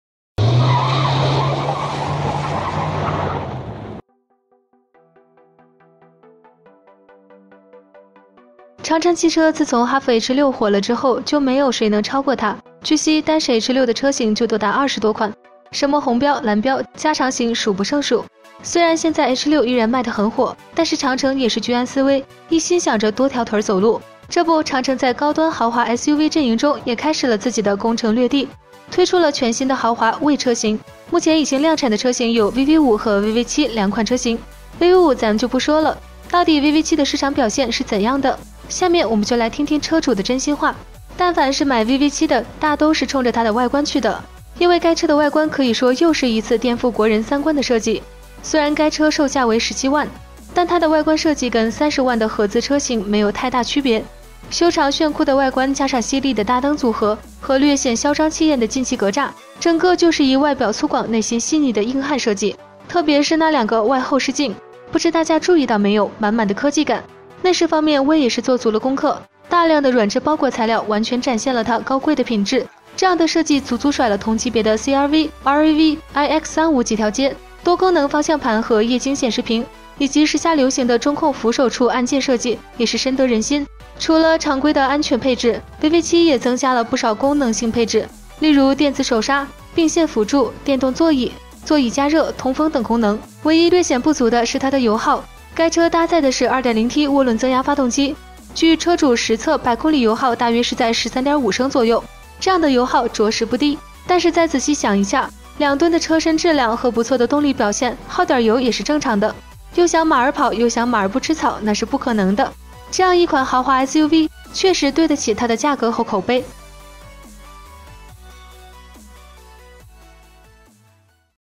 車評人都在誇VV7，實際表現到底如何？只有車主知道
雖然現在H6依然賣的很火，但是長城也是居安思危一心想著多條腿走路，這不長城在高端豪華SUV陣營中也開始了自己的攻城略地，推出了全新的豪華WEY車型，目前已經量産的車型有VV5和VV7兩款車型。VV5咱就不說了，到底VV7的市場表現是怎樣的，下面我們就來聽聽車主的真心話。